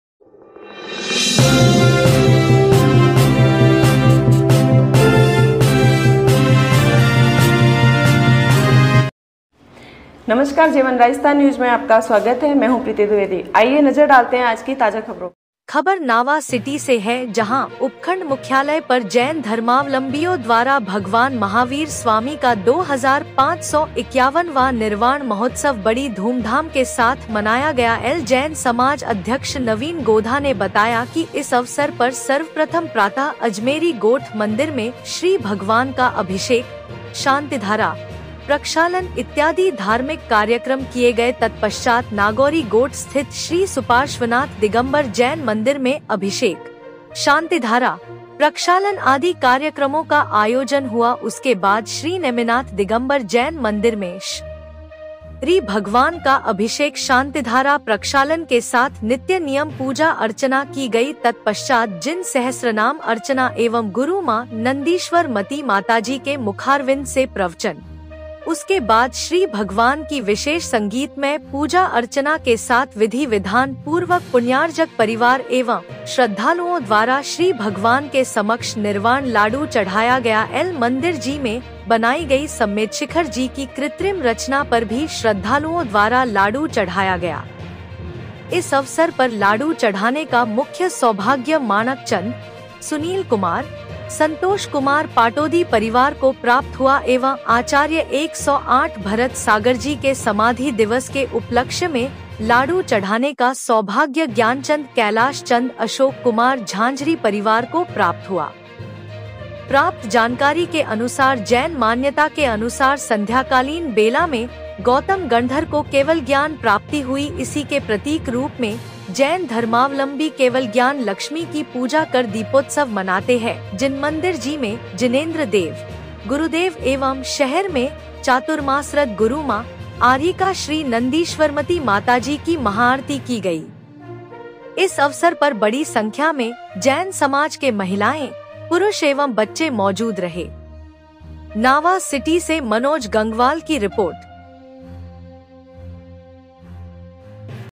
नावा सिटी। भगवान महावीर स्वामी का निर्वाण महोत्सव धूमधाम के साथ मनाया।
@ मनोज गंगवाल